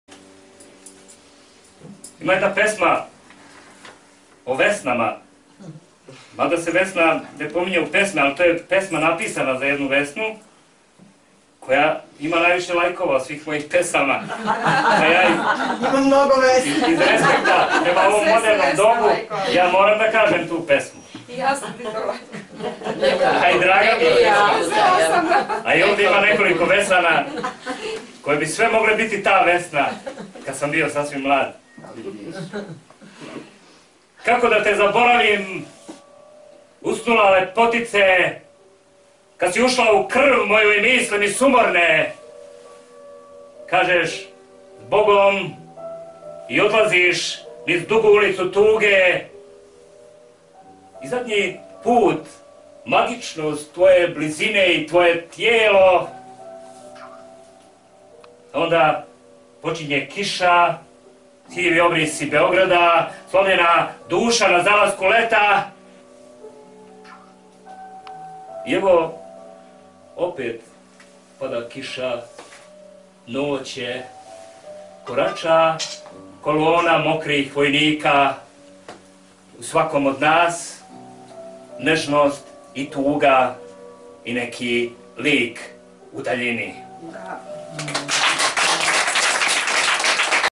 Nedjo Ćićo Stojanović - Pesma za jednu Vesnu - Mojih prvih 50 - poezija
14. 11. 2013. god. Bogotražitelj, Beograd    Pesma iz knjige Godine na struku trave kojoj sam zaboravio ime - Neđo Ćićo Stojanović